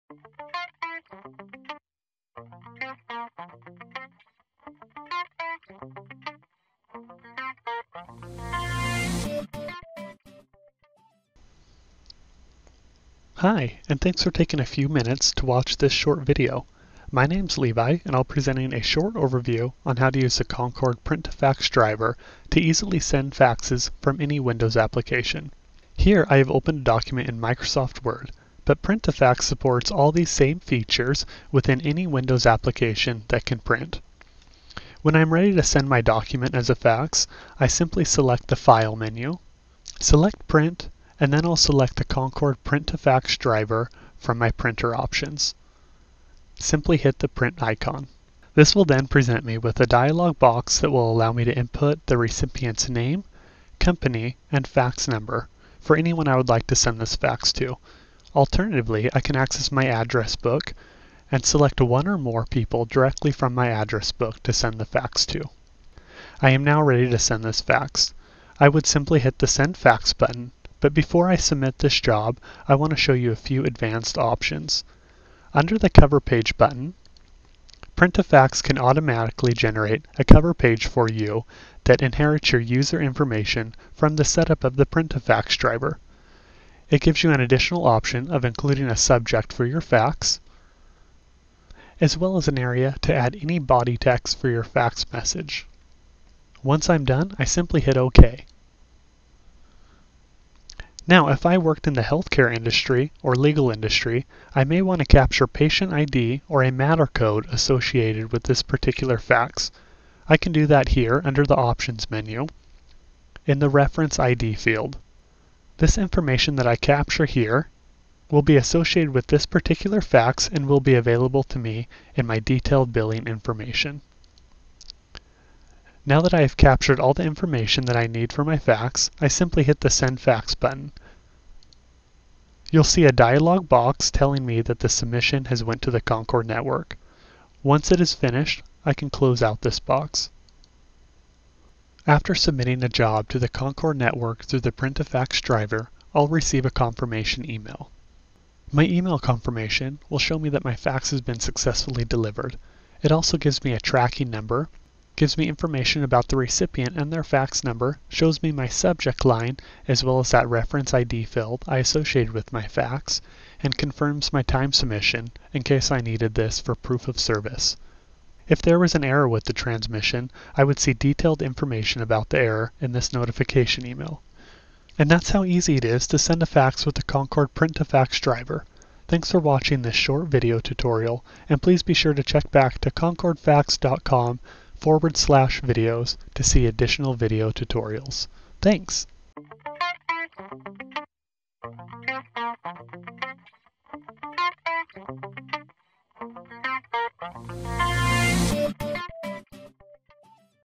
Print2Fax Driver from Concord Fax
- This short instructional video shows how to use the Print2Fax capability to send faxes on the Concord Fax Online platform. Concord Print2Fax is a utility designed for use with Concord Fax Online Email to Fax for sending faxes, but it can also be used for direct fax submission using a secure HTTPs connection. This utility allows one-click faxing from any windows application that supports a print function.

If you are not a current Concord Fax Client - Sign Up for a Free 30 Day Trial